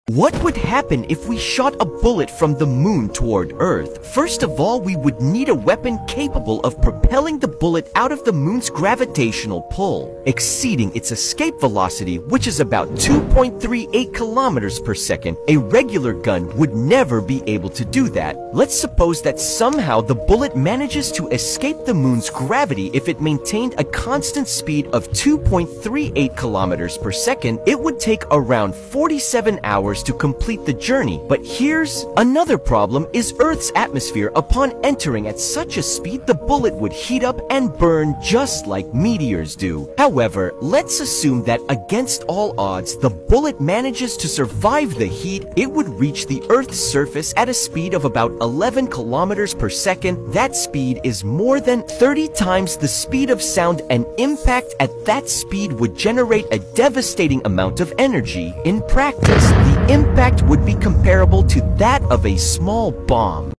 What Happens If You Shoot a Bullet from the Moon to Earth?
“What Happens If You Shoot a Bullet from the Moon to Earth? 🚀💥 | Space Physics Explained”
Credits: The Outliners

Ever wondered what happens if you fire a bullet from the Moon toward Earth? 🤔 In this mind-blowing space physics experiment, we break down the science behind shooting a bullet in outer space! 🌌 Will it reach Earth? What role do gravity, air resistance, and velocity play in this cosmic scenario? Find out now!